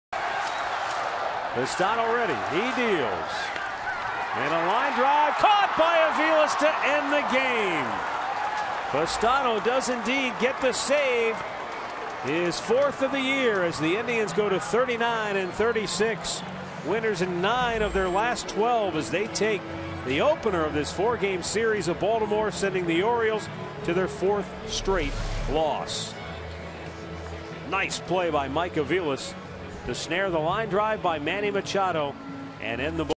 Aviles' leaping snare secures Pestano's save
6/24/13: Mike Aviles climbs the ladder at short to make a great catch on Manny Machado's liner for the final out of the Indians' 5-2 win